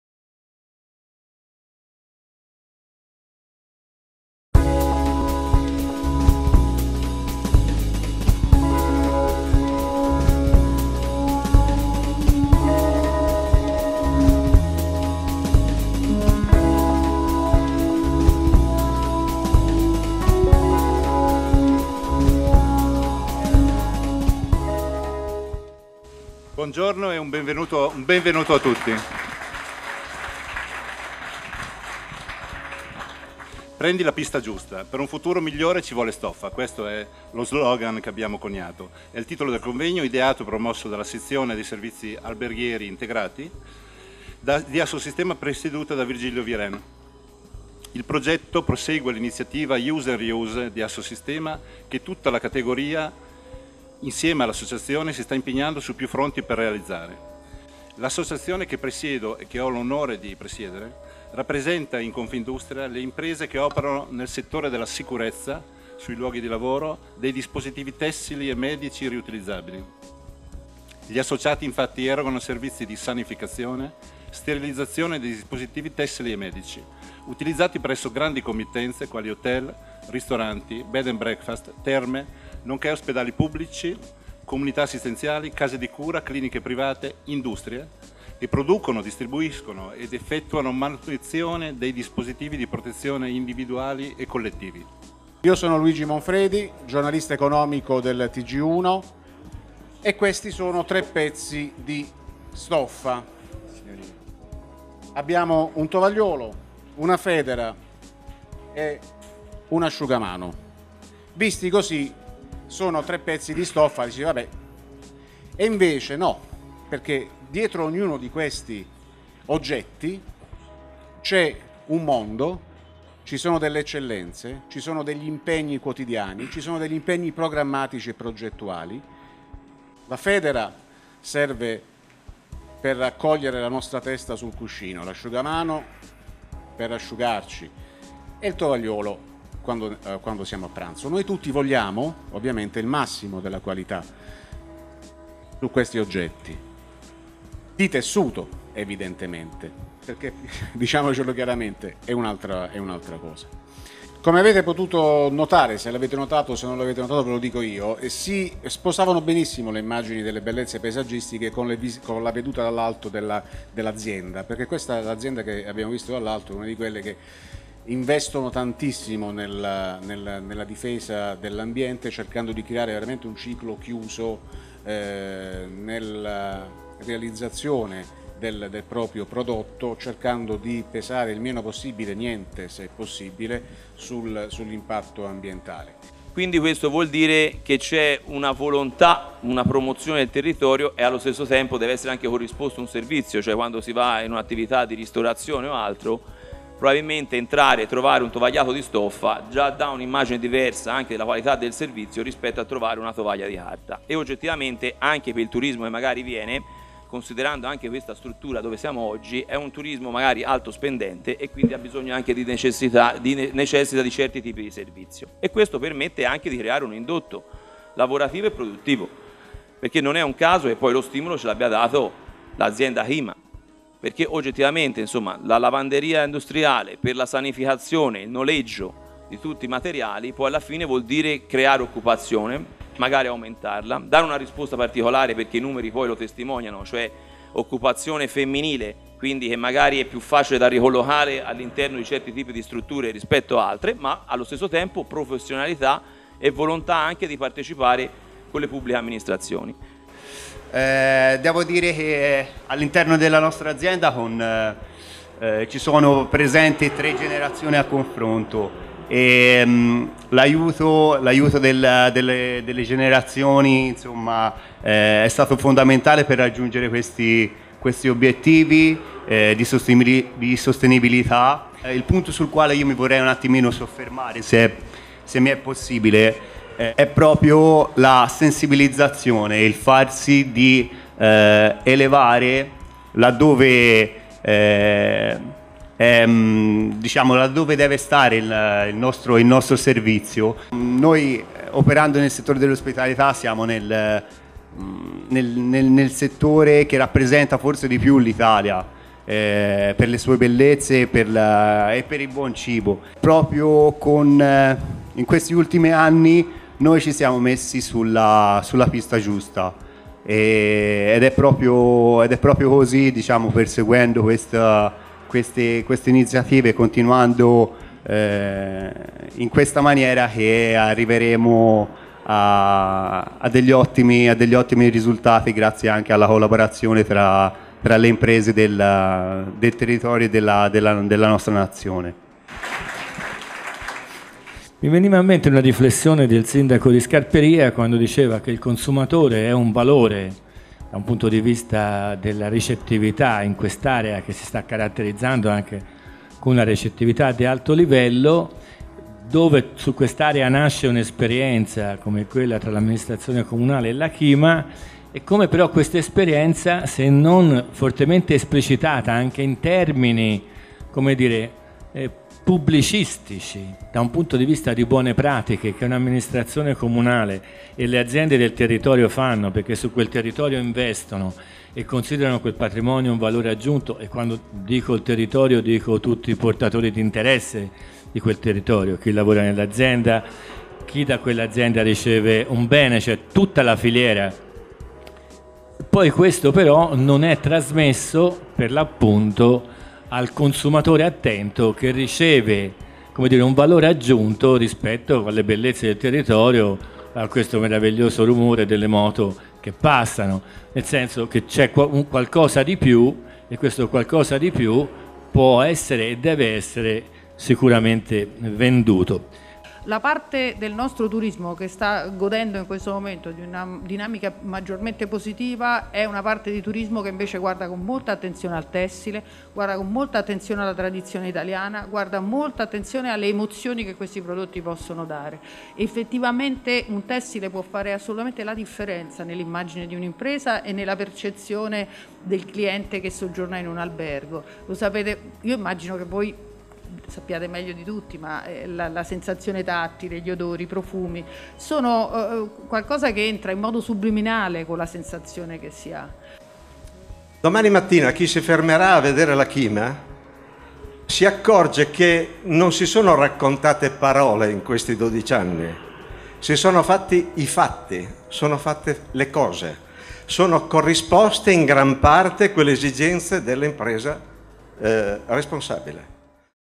Convention Assosistema 12.05.2017
Stoffa al posto della carta. Il Mugello fa scuola, se ne parla in autodromo. Grande successo di pubblico all’evento “Prendi la pista giusta: per un futuro migliore ci vuole stoffa!”, organizzato da ASSOSISTEMA e ASSOFORNITORI venerdì 12 maggio 2017 all’Autodromo del Mugello, con l’obiettivo di promuovere l’utilizzo del tessile nel settore della sanificazione per le strutture alberghiere e della ristorazione.